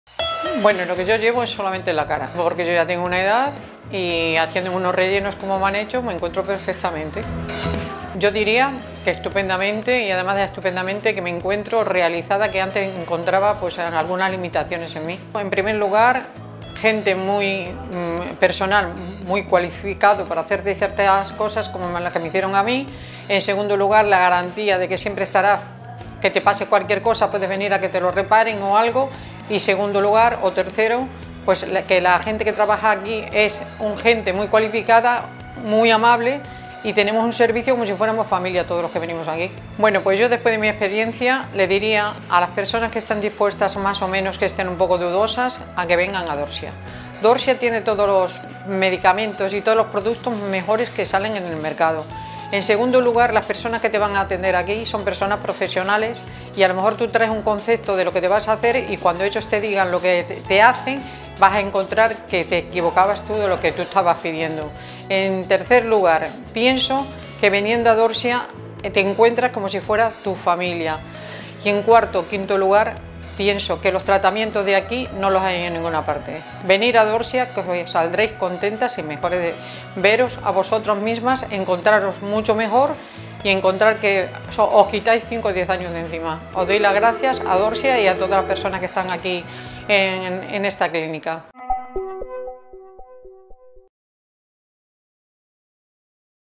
Manuela Caballero - Medicina facial
Desde que pasó por la clínica Dorsia de Lorca, Manuela siente que se ha quitado años de encima. Destaca la profesionalidad de todo el personal que la atendió y valora positivamente el asesoramiento recibido a la hora de elegir el tratamiento que mejor se adecuaba a su tipo de belleza.